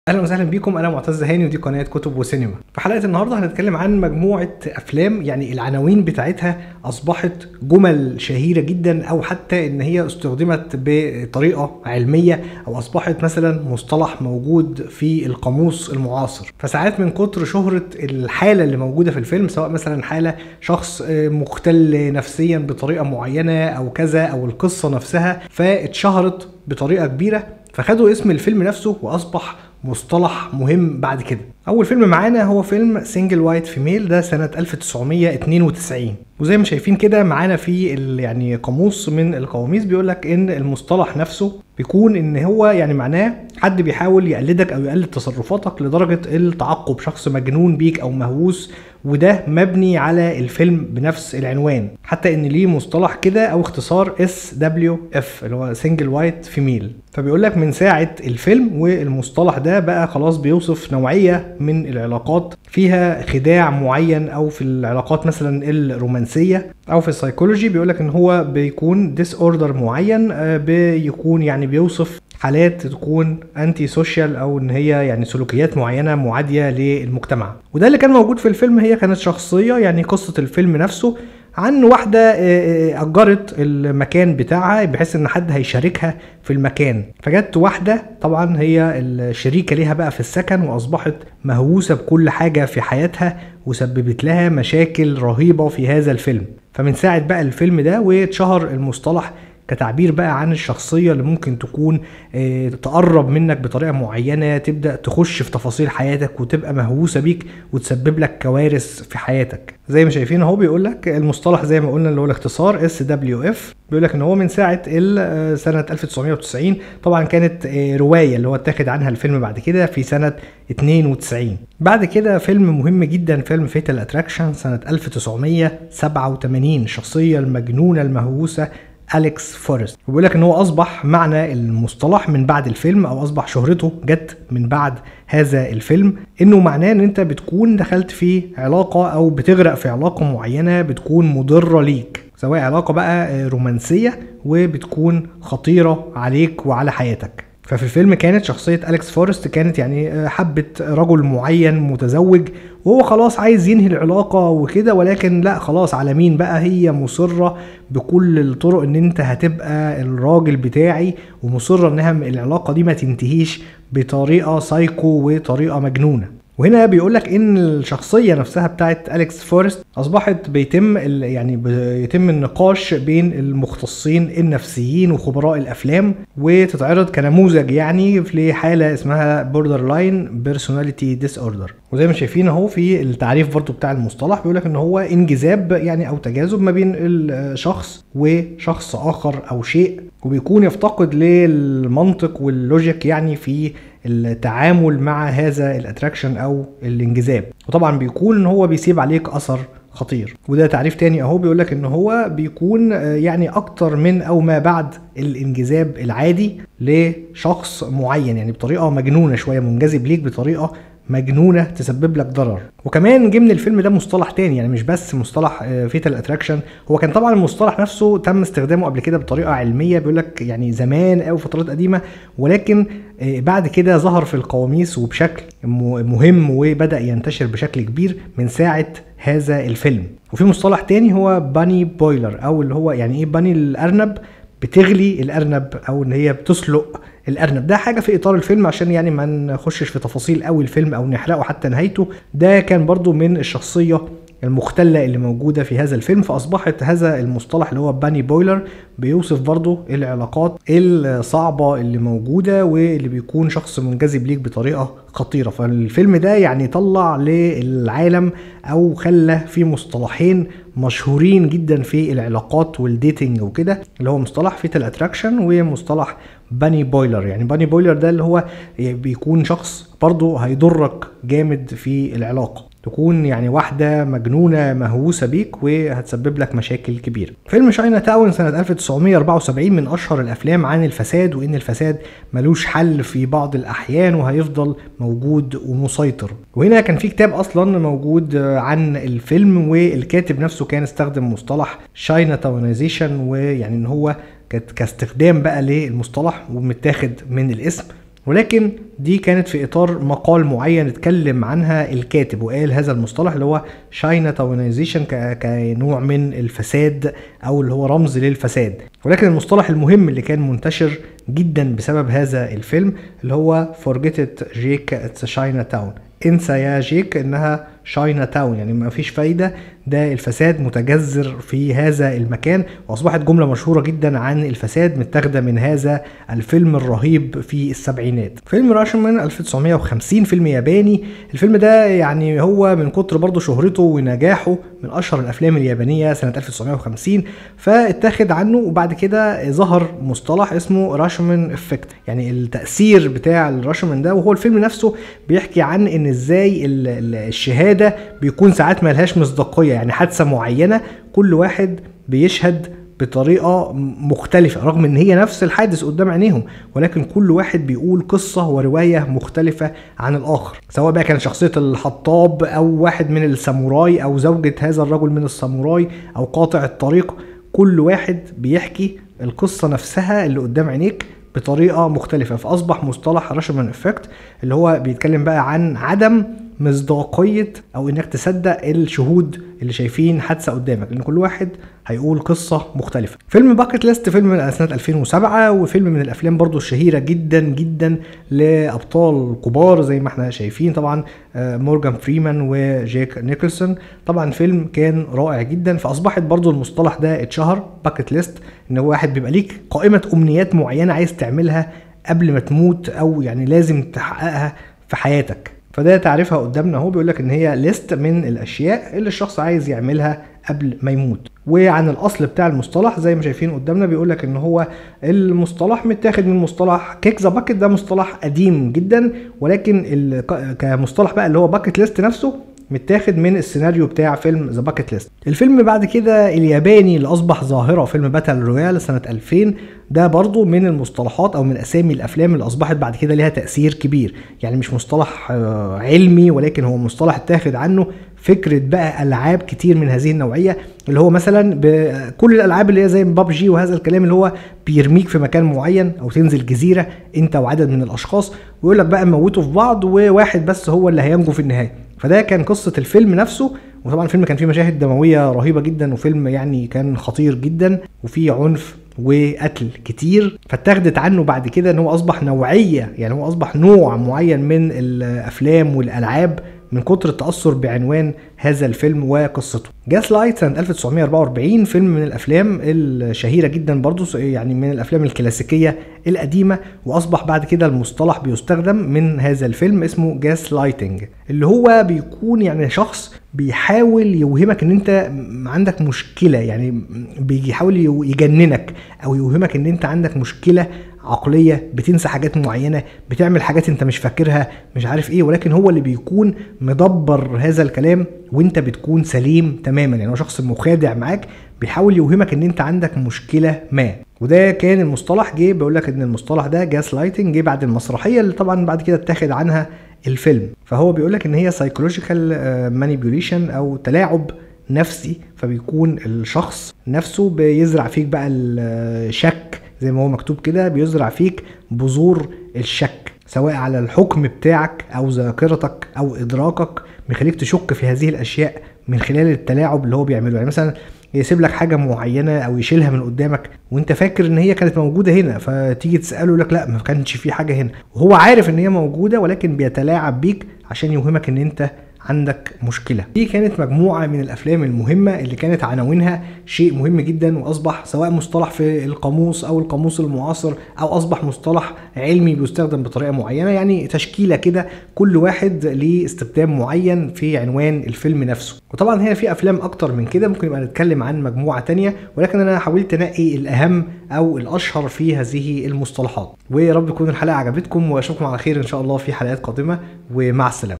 عناوين أفلام تحولت لمصطلحات مهمة
بعض عناوين الأفلام التي أصبحت موجودة في القواميس أو يتم تداولها بين خبراء علم النفس أو أصبح لها تأثير كبير.